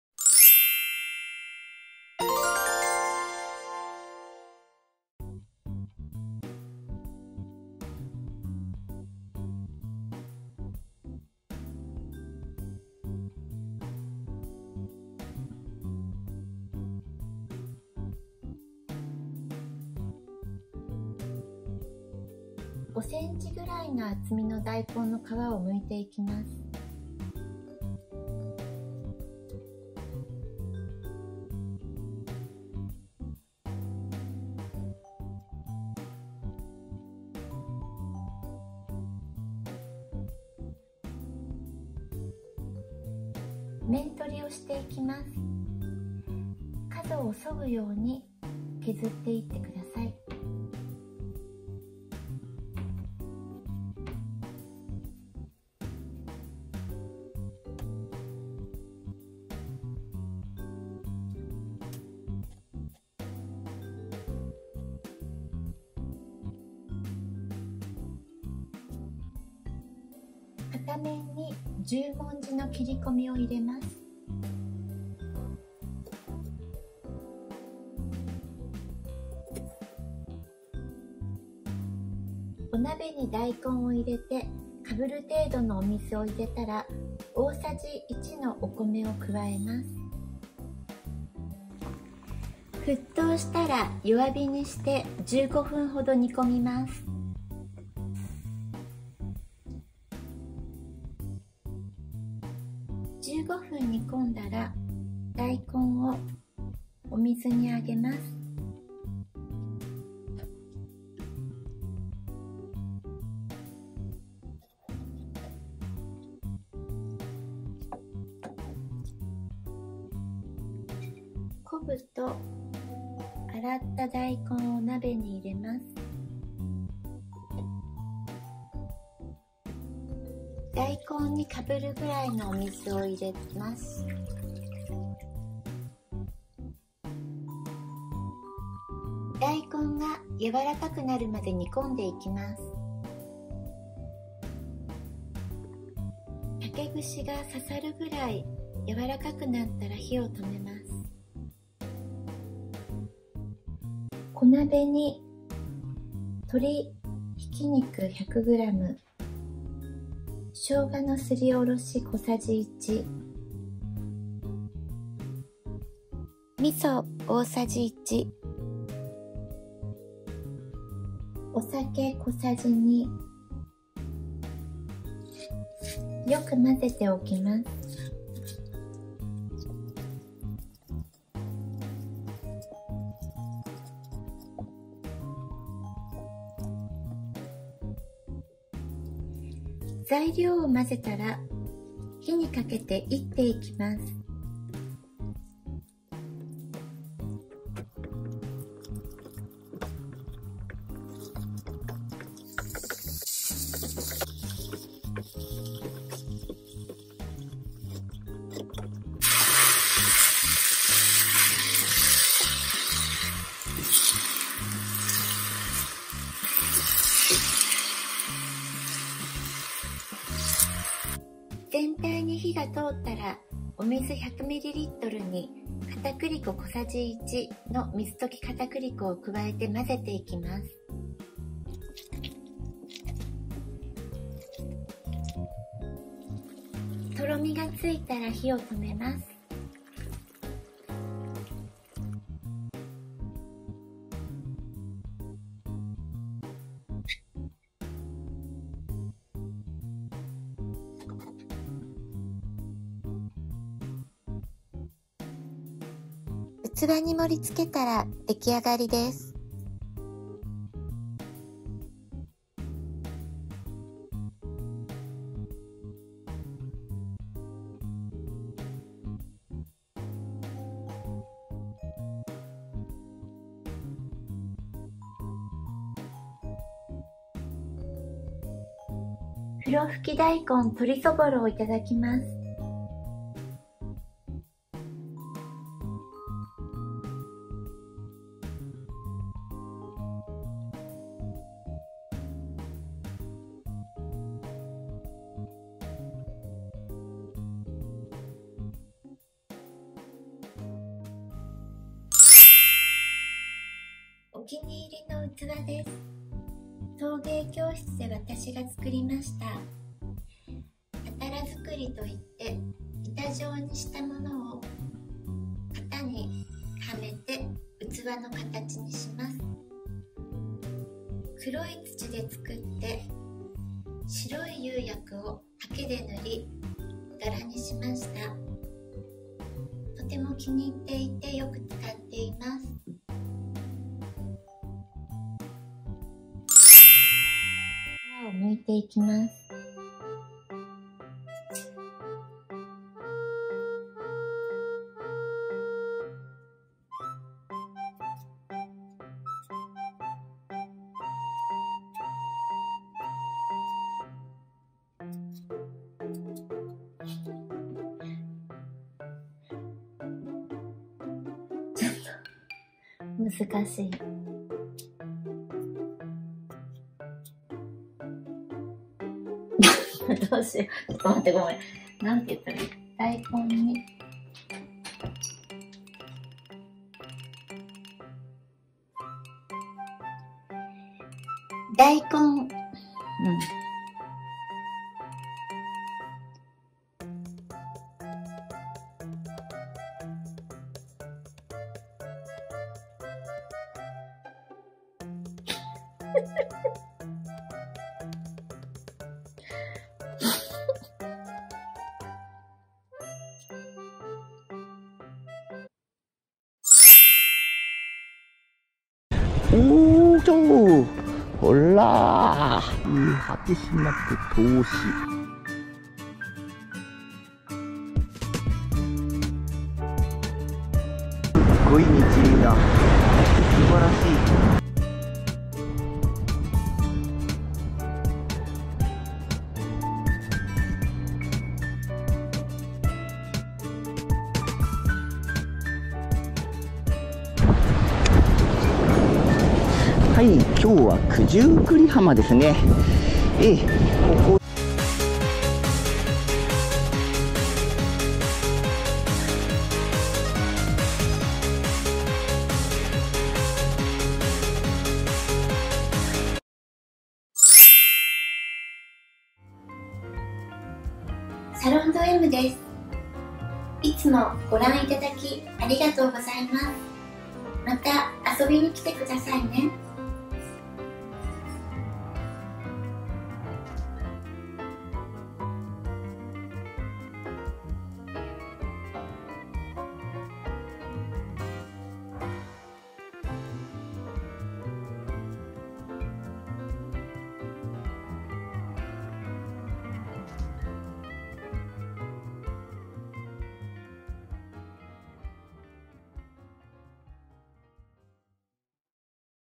美味しい ふろふき大根の作り方 鶏そぼろ味噌かけ
美味しいふろふき大根の作り方　鶏そぼろ味噌かけ

00:00　Let's cooking
00:14　レシピ
00:22　ふろふき大根の作り方
02:32　鶏そぼろ味噌の作り方
05:04　私のお気に入り
05:38　NGの女王登場
06:49　釣りクックfukufukumanさんご紹介